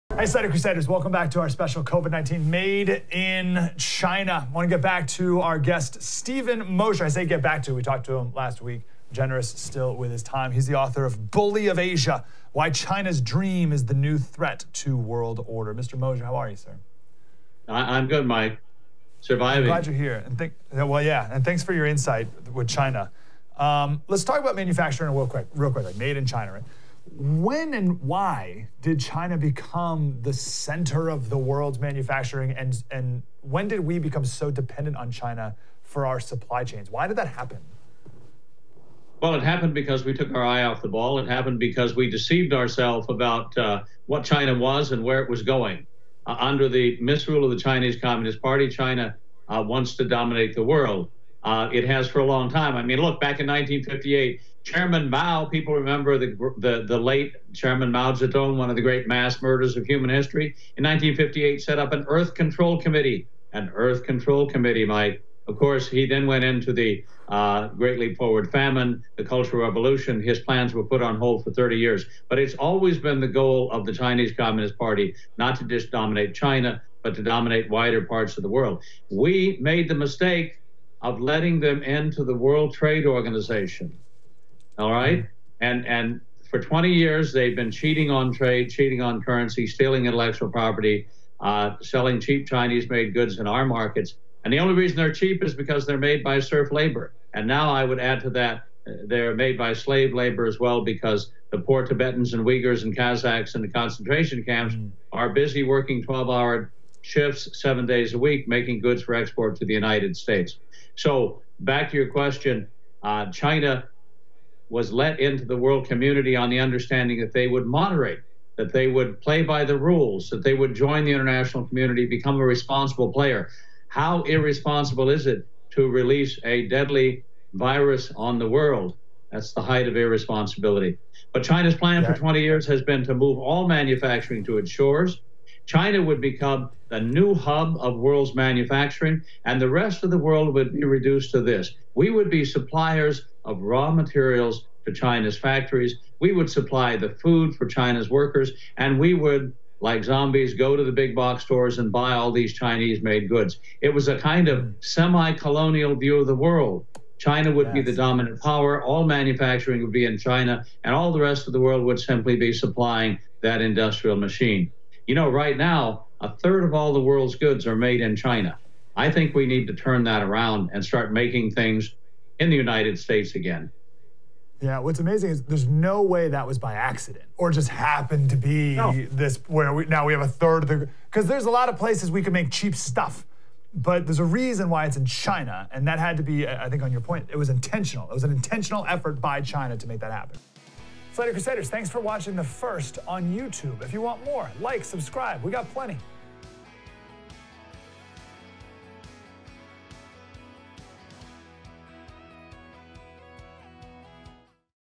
INTERVIEW: Stephen Mosher On China's Plans To Dominate The World
Mike Slater speaks to author Steven Mosher about China's grand plan to dominate the global economy and subjugate the USA.